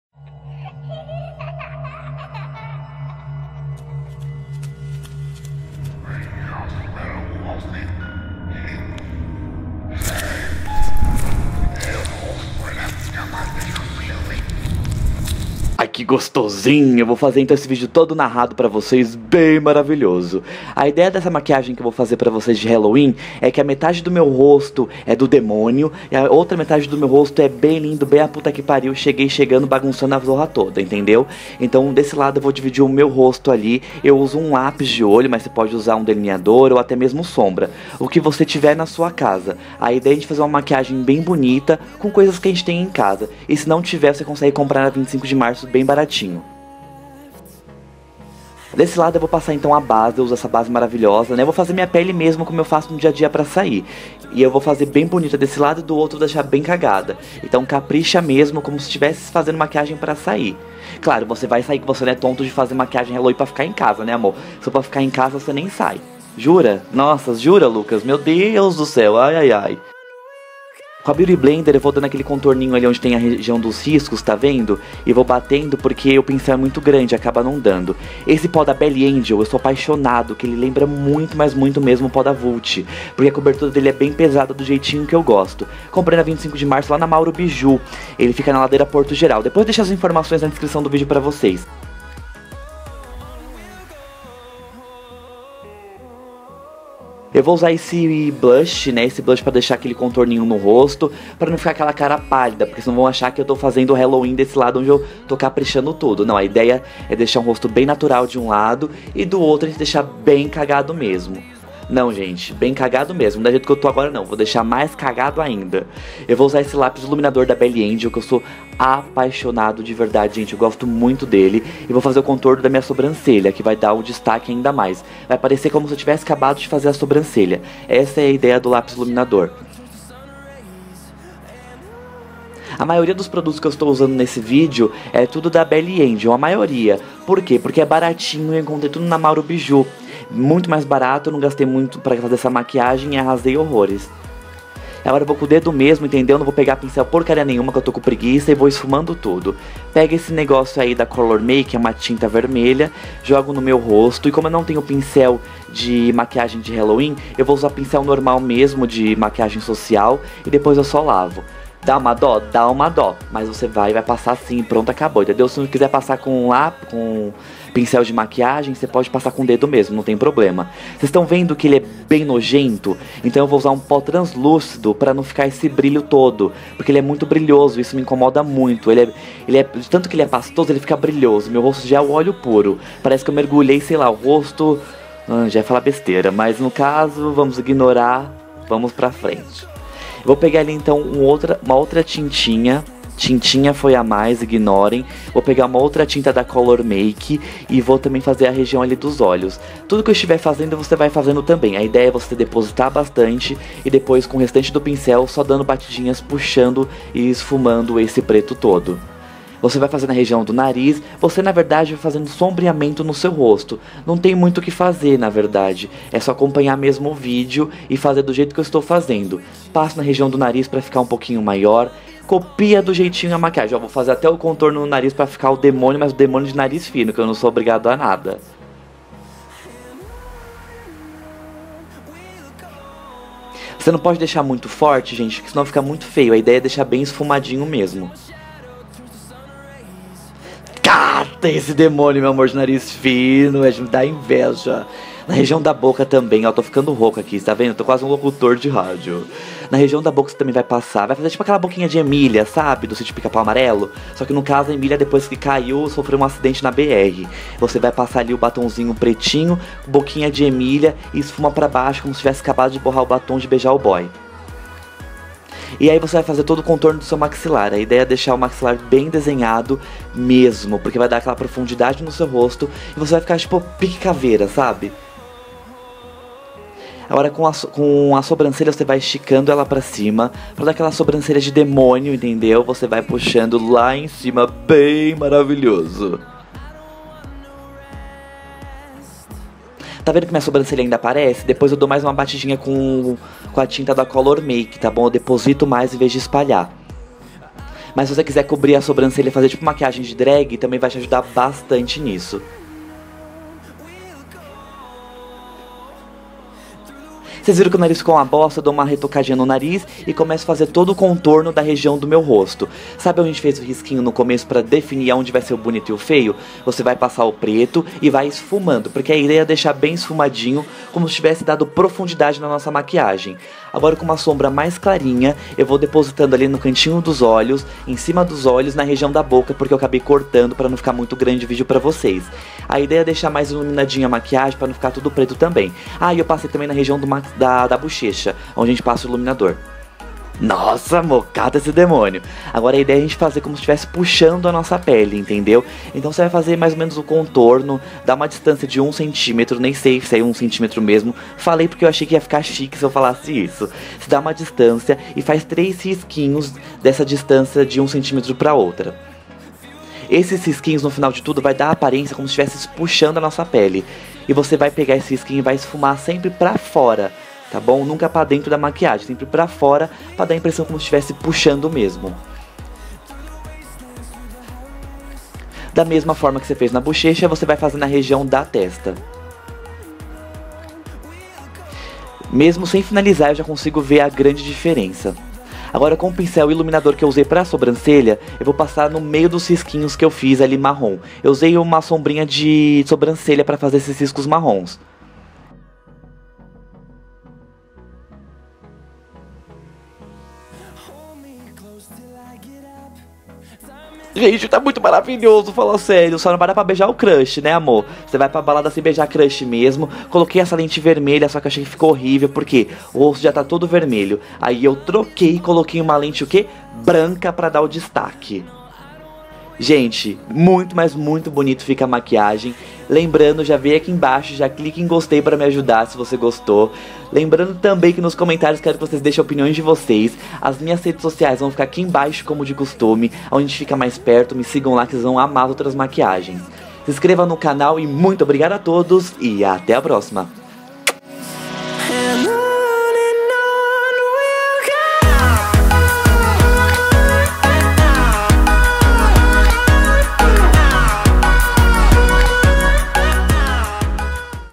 Maquiagem halloween: DEMÔNIO VS PRÍNCIPE - UMA RAPIDINHA
Você não precisa ser craque em maquiagem para fazer essa MAQUIAGEM INCRÍVEL, é super facinho, vem comigo!

CUPOM 10%: UMARAPIDINHADOLUCAS
Com esse cupom não tem frete, dá pra arrasar!

JOB | CONTATO | ASSESSORIA: Greyce Lidiane